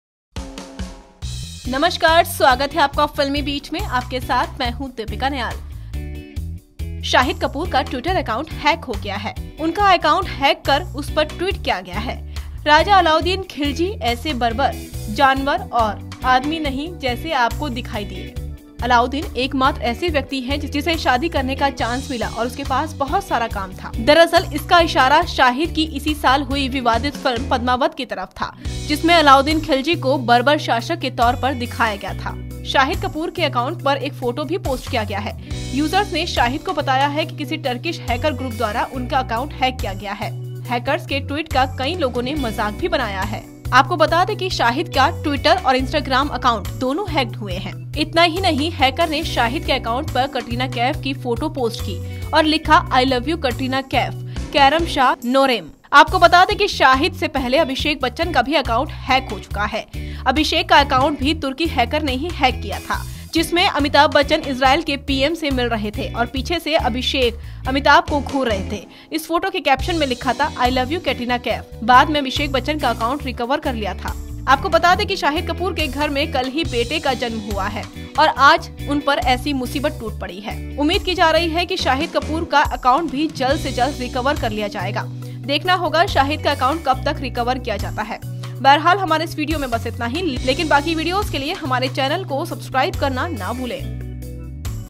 Shahid Kapoor's Twitter & Instagram accounts HACKED | FilmiBeat
Actor Shahid Kapoor's Twitter and Instagram accounts were hacked on Thursday afternoon and appear to have been restored now. On his Instagram story, Shahid Kapoor posted this message for his fans: "Hey guys, thank you for the wonderful wishes. My Instagram and Twitter had been hacked. Watch the video to know more!